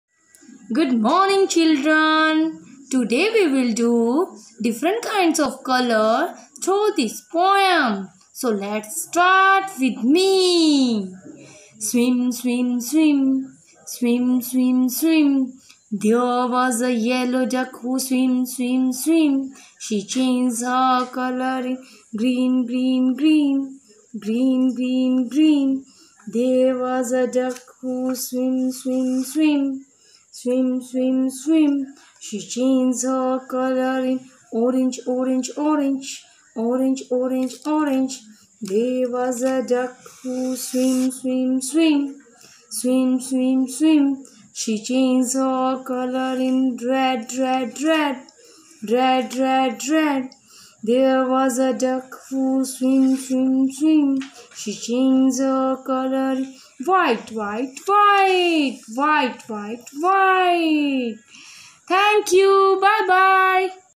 Color activities
A duck changing different kinds of color.